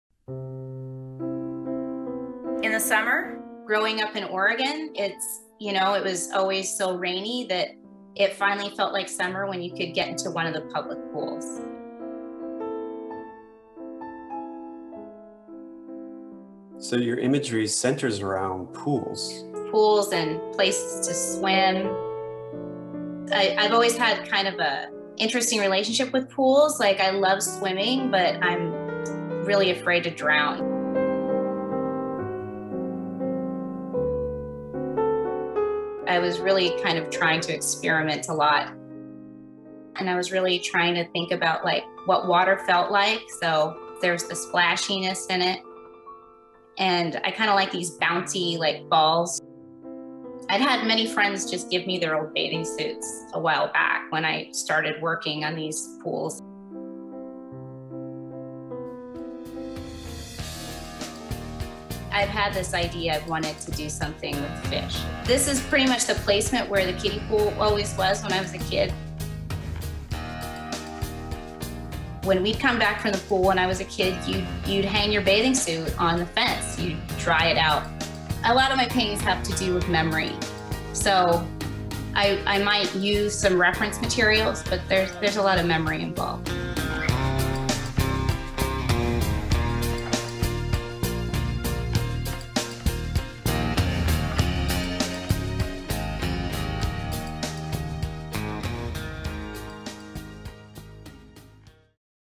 Painter Susan Sullivan talks pools & swimming
Artist & painter Susan Sullivan speaks about her recent exhibition, which was on view June 30 - July 25, 2021, at Cerulean Arts Gallery, Philadelphia.